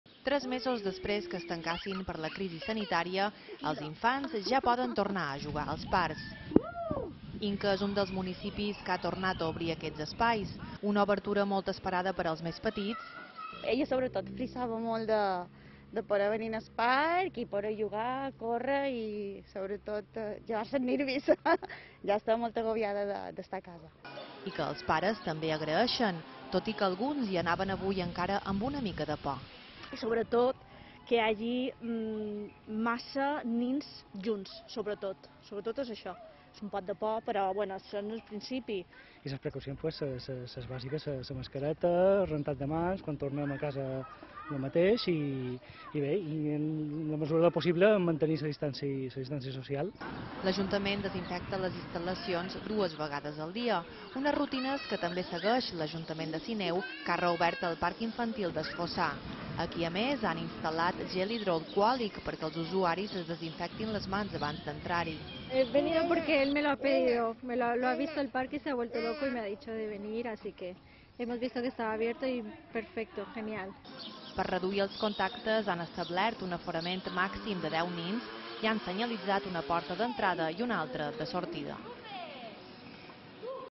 Reobren els parcs infantils
De mica en mica torna l’activitat als parcs infantils. 3 mesos després que es tancassin per la crisi sanitària, els infants ja hi poden tornar a jugar. Inca és un dels municipis que ha tornat a obrir aquests espais. Una obertura molt esperada per als més petits i que els pares també agraeixen.Tot i que alguns hi anaven avui encara amb una mica de por.

L’Ajuntament d’Inca desinfecta les instal·lacions dues vegades al dia. Unes rutines que també segueix l’Ajuntament de Sineu que ha reobert el parc infantil des Fossar. Aquí, a més, han instal·lat gel hidroalcohòlic perquè els usuaris es desinfectin les mans abans d’entrar-hi. Per reduir els contactes, han establert un aforament màxim de 10 nins i han senyalitzat una porta d’entrada i una altra de sortida.